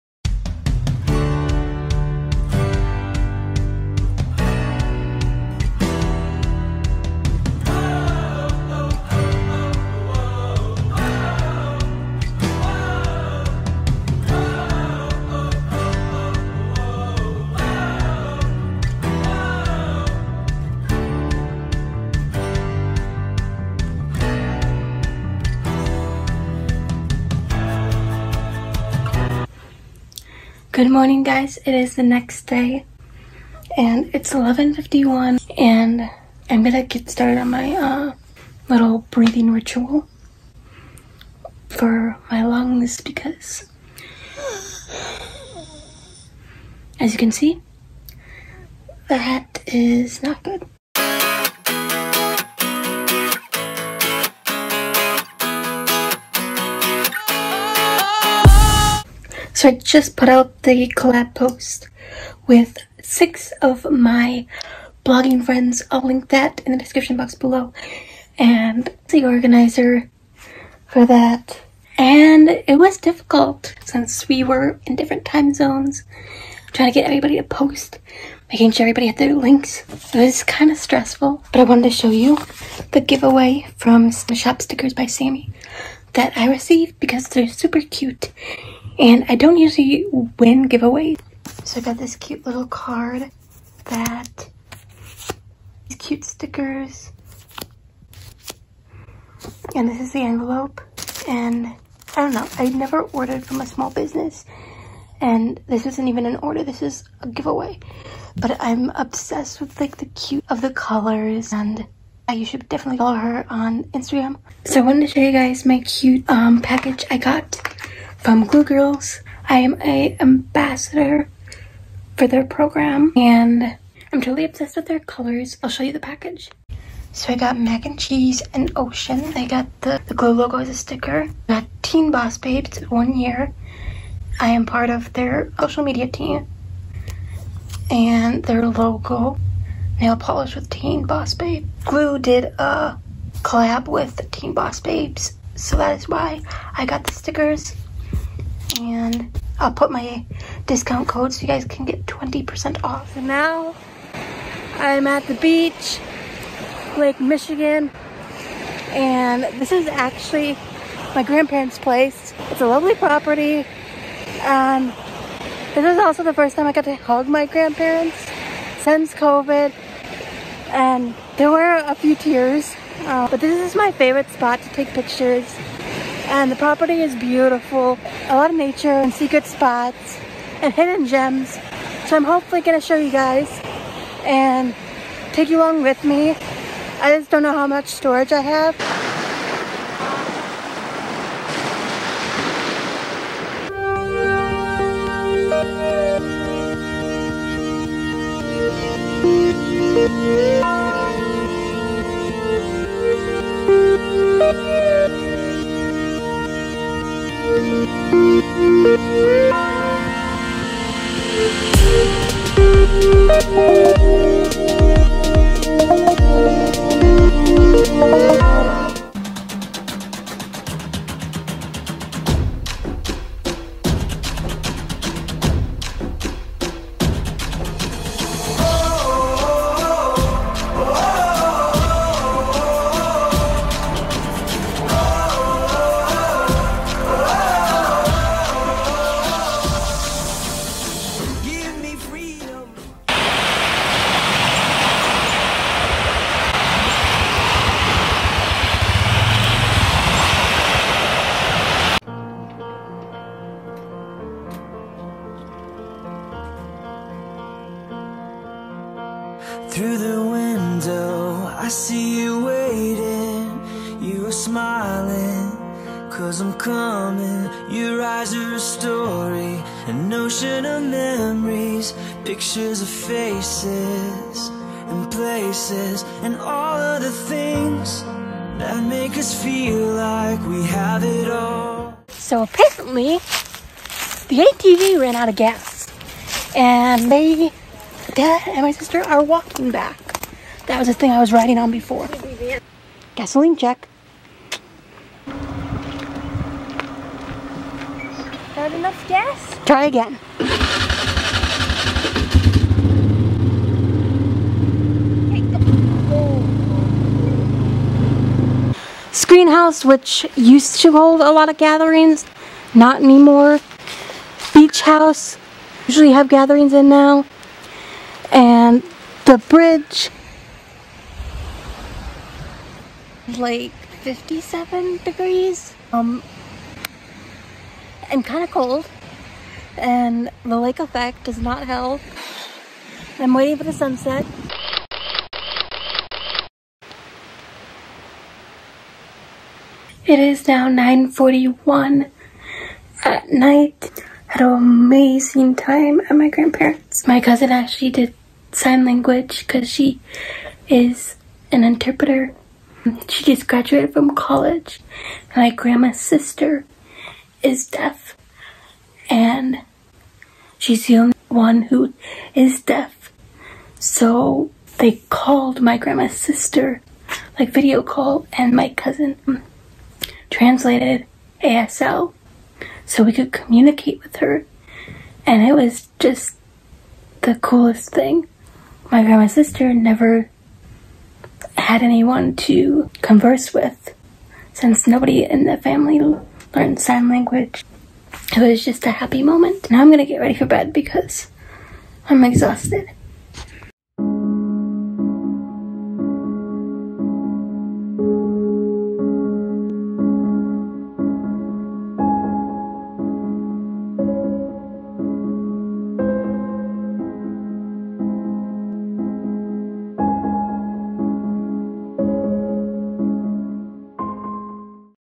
Vlog : 5/30/21 Beach Adventure!
Welcome to the beach! Let me take you along on a fun adventure at my grandparent's beach property! Complete with hidden gems and nature property!

-Buy stickers like the ones from my video on the Instagram account
Tools & Equipment:
Adobe Premiere Rush CC:
Studio Light /w White Umbrella Reflector - 6500 K Light Bulb